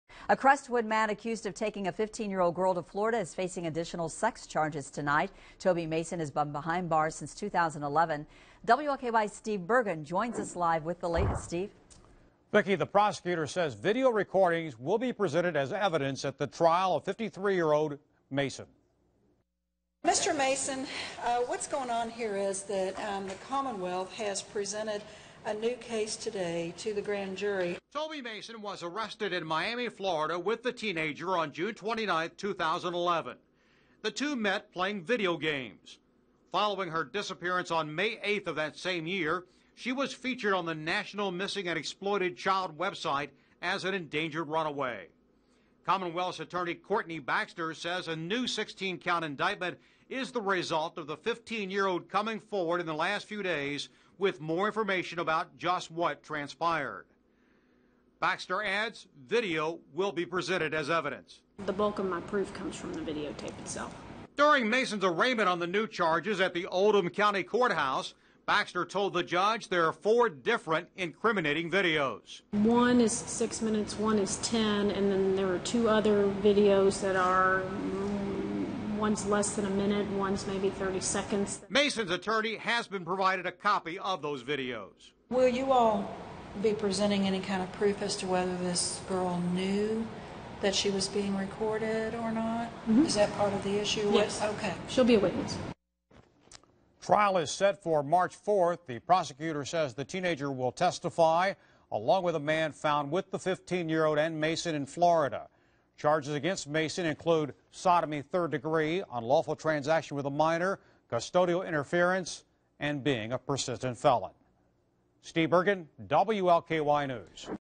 New charges added in case of man found in Fla. with teen
A Crestwood man accused of taking a 15-year-old girl to Florida faces additional sex charges.